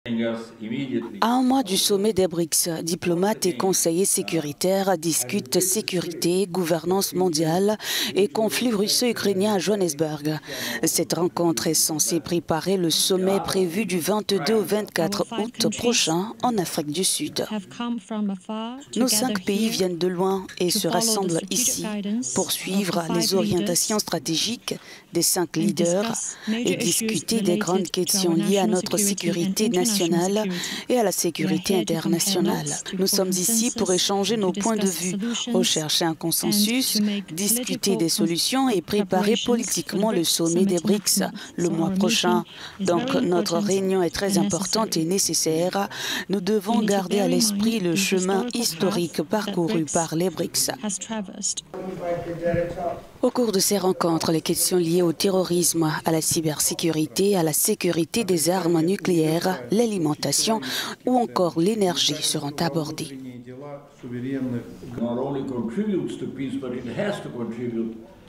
Afrique du Sud : les BRICS se penchent sur la sécurité mondiale
Les préoccupations sécuritaires des pays africains et la gouvernance multilatérale mondiale seront également discutées en préparation du sommet.